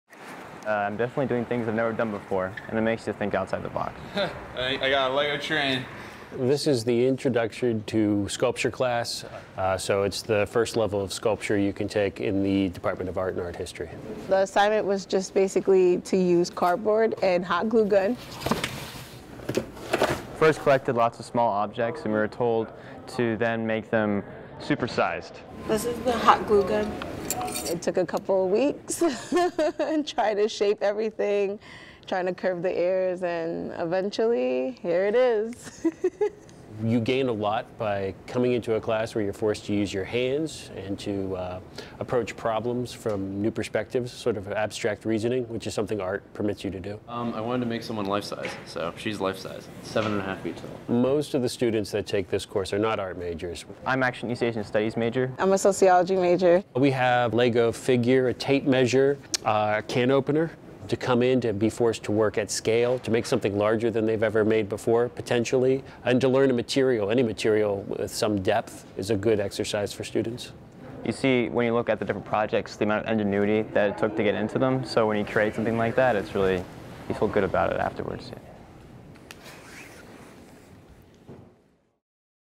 From Cardboard to Art: Larger than Life Sculptures
Sixteen Dickinson College students in a variety of majors and with little to no art expertise come together in Fundamentals of Sculpture and Three-Dimensional Design, a course taught by Anthony Cervino that truly encapsulates the meaning of "interdisciplinary." The students were tasked with taking a small object and turning it into an oversized sculpture using nothing but cardboard and hot glue.